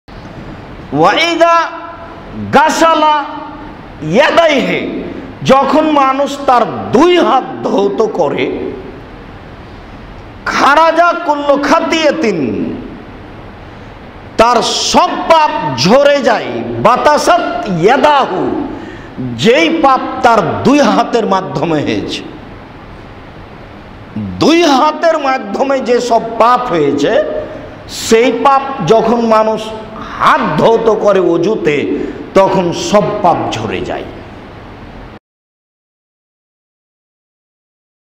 কি করলে দুই হাতের সব পাপ ঝরে যায় | শায়খ আব্দুর রাজ্জাক বিন ইউসুফ ওয়াজ Abdur Razzak Bin Yousuf Waz
কি করলে দুই হাতের সব পাপ ঝরে যায় | শায়খ আব্দুর রাজ্জাক বিন ইউসুফ ওয়াজ | Shaikh Abdur Razzak Bin Yousuf Waz